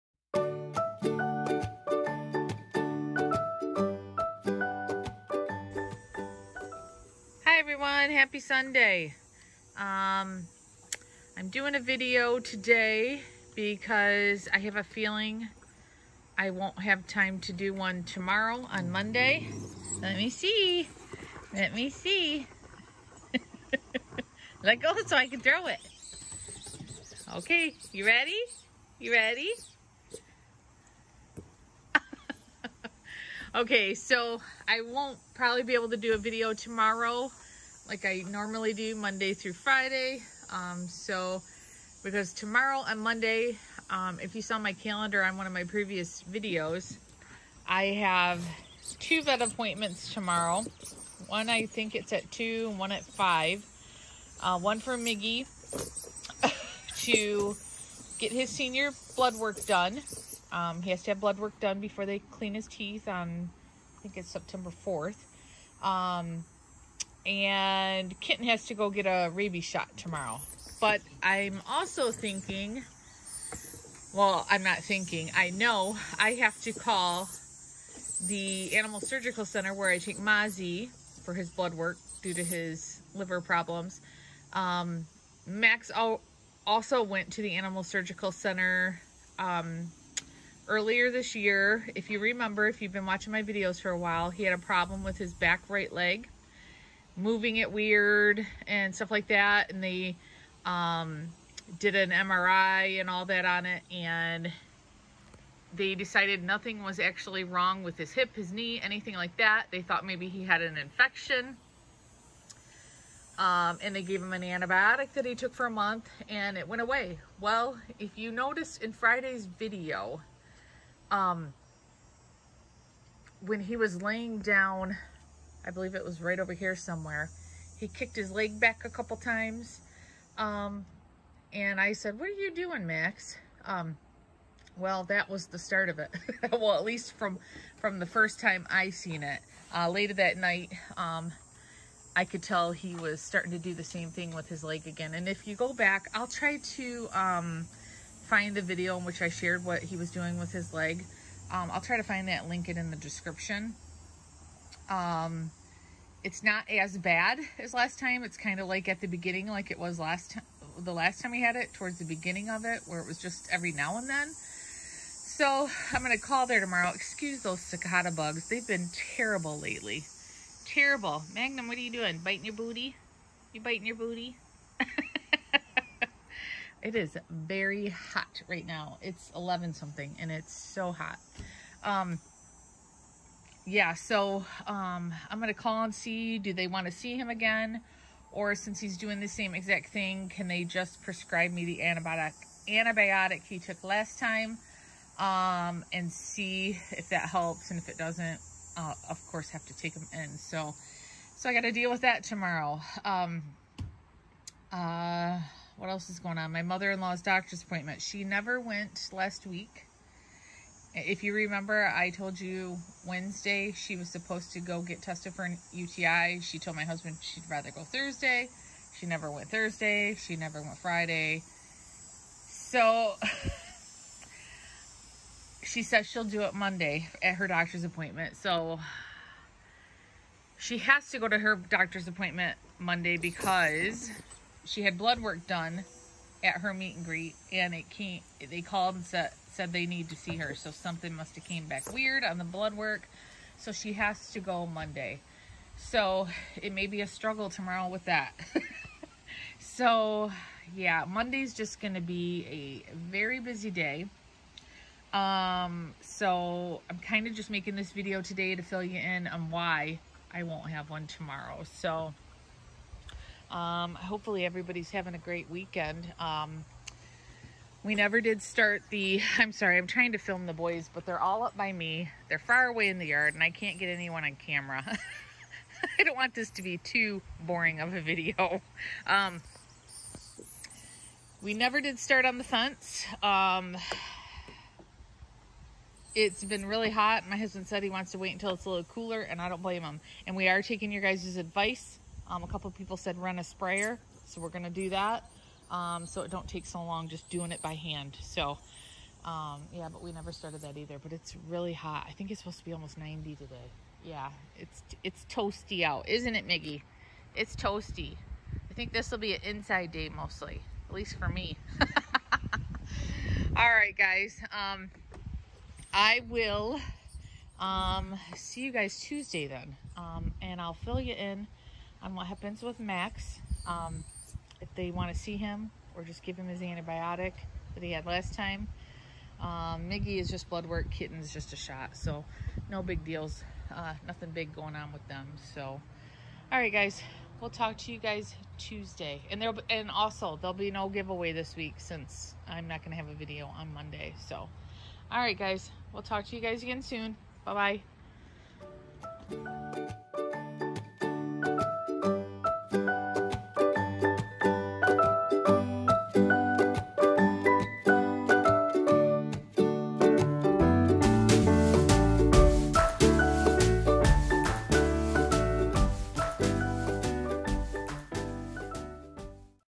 No Video Tomorrow | Life With 6 Schnauzers
Here's the video that has a clip in it showing what was going on with Max's leg back in April.  It's the same thing that is happening now.  It start's around the 8:30 mark on the video.

Kim, Mozzie, Max, Marley, Miggy, Magnum, & Milo